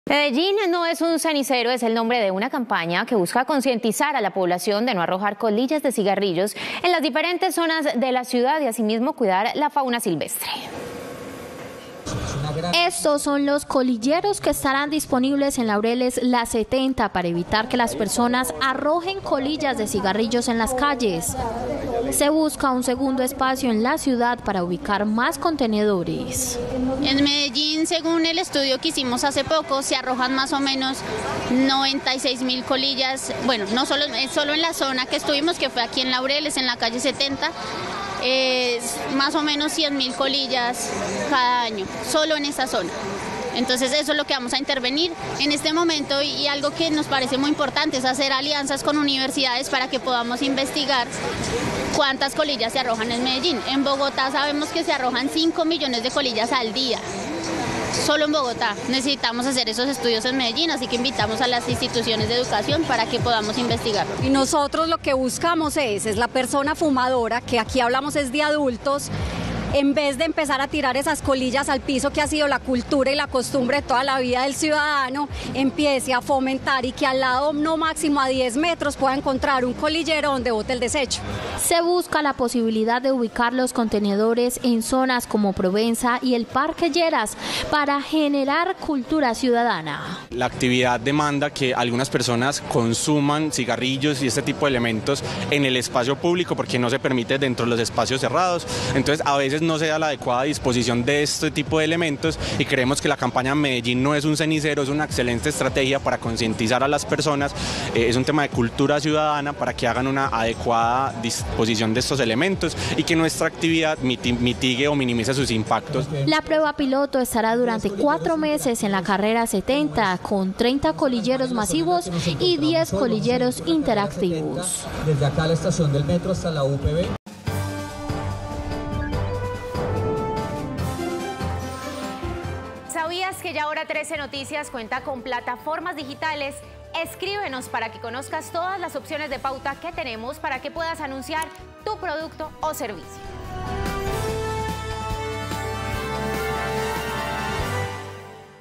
“Medellín no es un cenicero”: la estrategia para evitar 100 mil colillas en el suelo de la calle 70
En Medellín se pretende concientizar a los fumadores con esta estrategia.